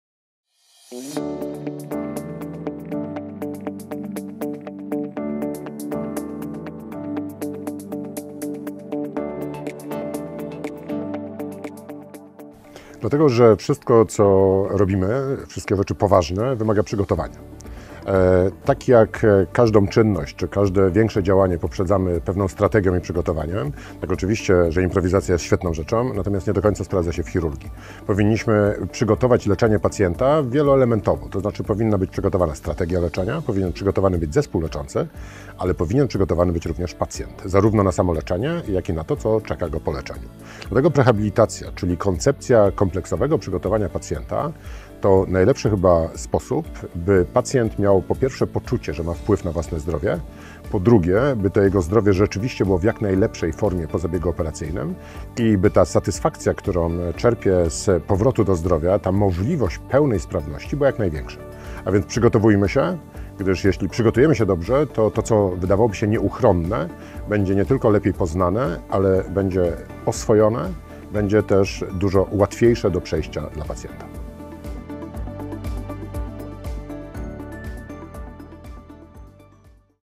Dlaczego prehabilitacja jest ważna dla pacjenta? - Prof. Tomasz Banasiewicz
Prehabilitacja to proces przygotowujący pacjenta do operacji czy długotrwałego leczenia. Pomaga w łagodniejszy sposób przejść przez zabiegi i skrócić czas rekonwalescencji. Posłuchaj, co na ten temat twierdzi prof. dr hab. n. med. Tomasz Banasiewicz.